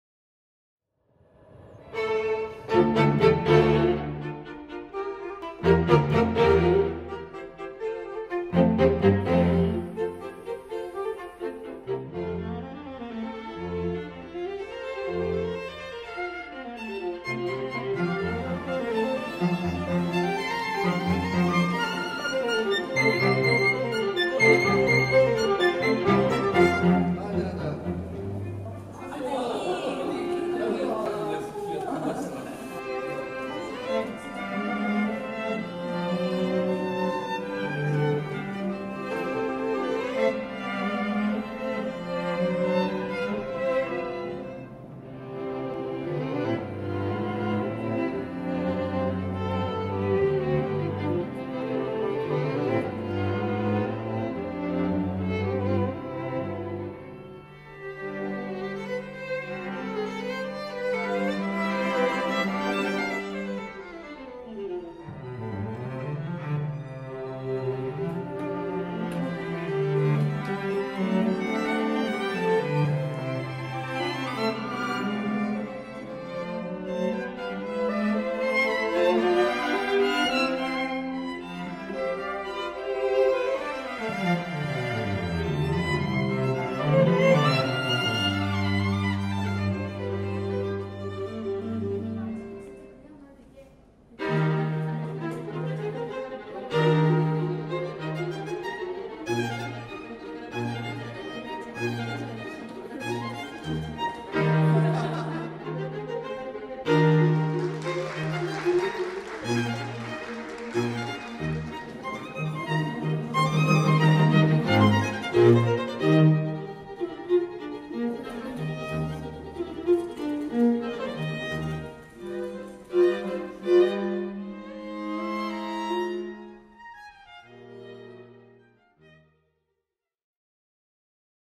The 5th Artsylvia Chamber Music Audition(2016)_String Quartet
Abel Quartet won the Artsylvia Prize at the 5th Artsylvia Chamber Music Audition.
(Sylvia Hall, Arts Center in Gimpo on January 30, 2016)

제5회 아트실비아 실내악 오디션(2016)_현악4중주
아트실비아상_아벨 콰르텟